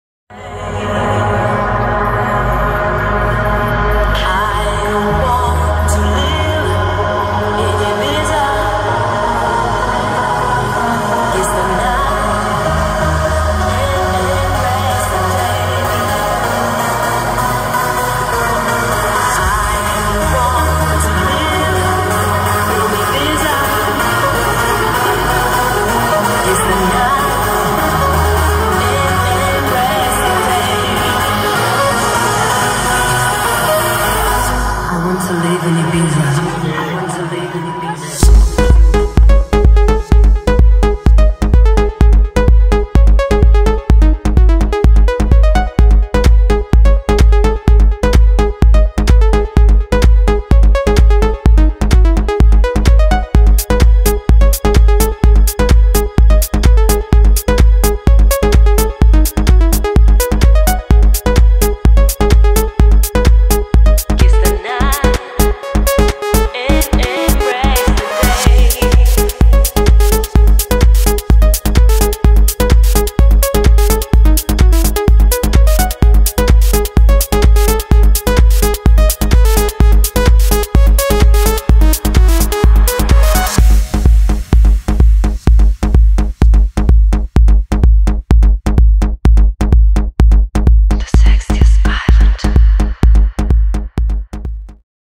sᴜɴsᴇᴛ ᴇᴍ ᴠɪʟᴀ ᴅᴏ ᴄᴏɴᴅᴇ - ᴍᴏᴍᴇɴᴛᴏ "ɪʙɪᴢᴀ ғᴏʀ ᴅʀᴇᴀᴍs" 🎶🎧🌅
Momento especial do Sunset do dia 15/06/2023, em Vila do Conde 🎶🎧

Em breve irei publicar um resumo do Sunset 🌅